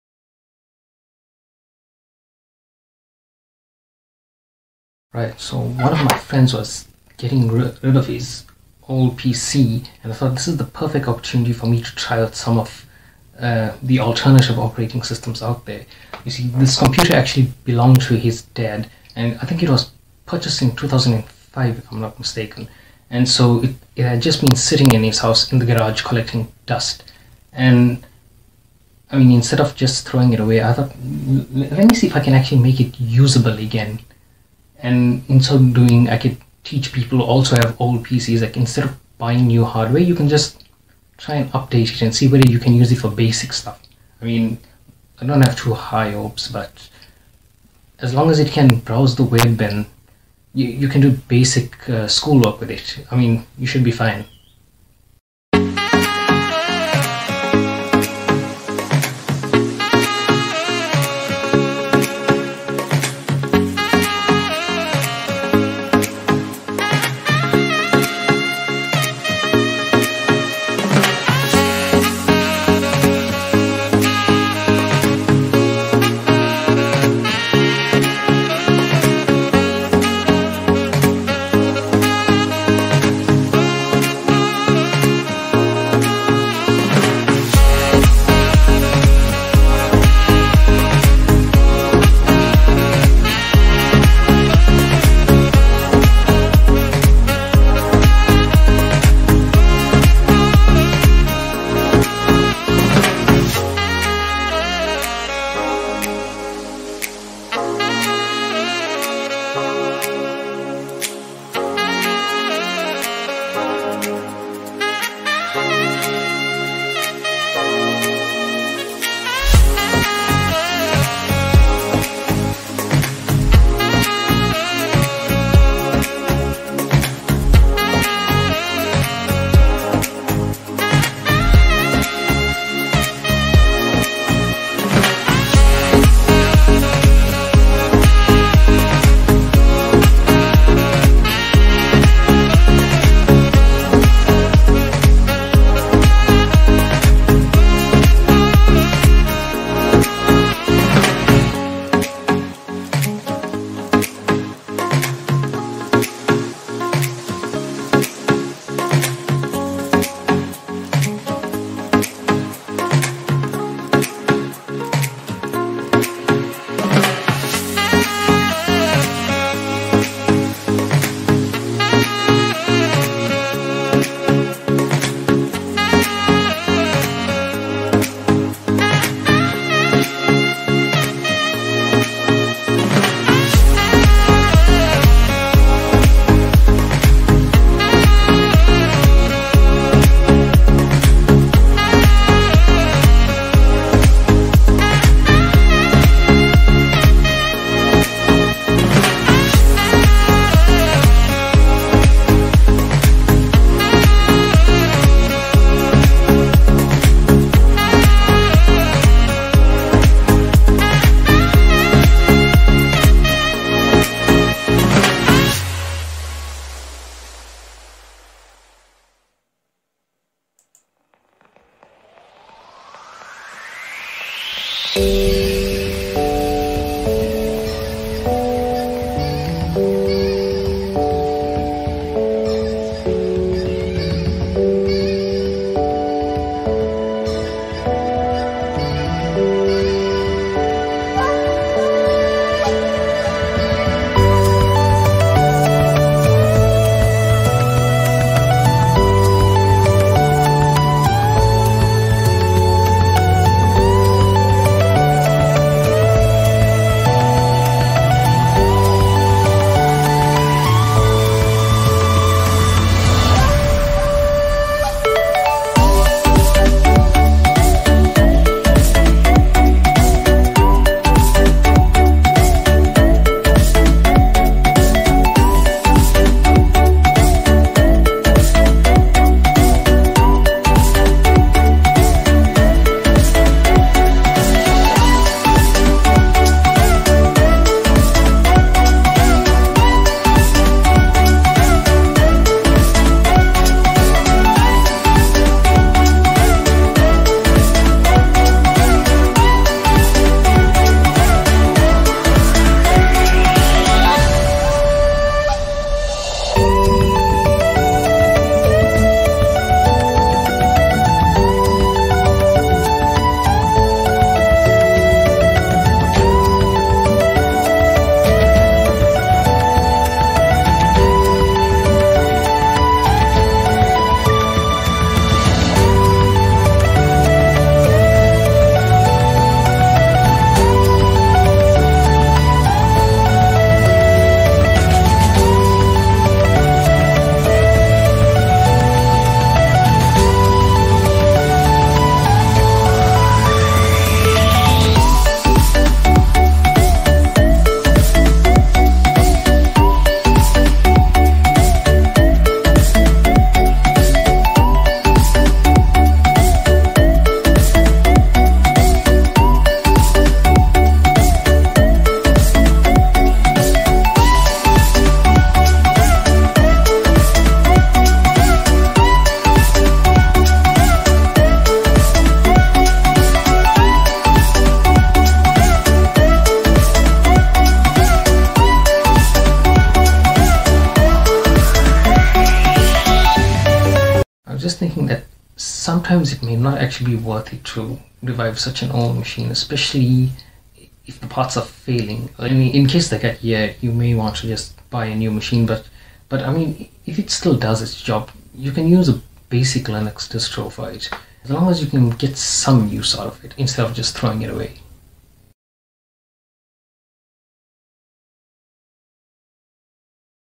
IT: Saving an old PC with Linux - Part 1
Whether you are on a tight budget or you own a small business, using Linux can help to extend the lifespan of your ageing hardware. In this part 1, I test for a suitable Linux distro.
I apologize for the poor video quality. It was edited on a low spec laptop (Core 2 Duo, 4GB RAM) so I had to tone down the settings.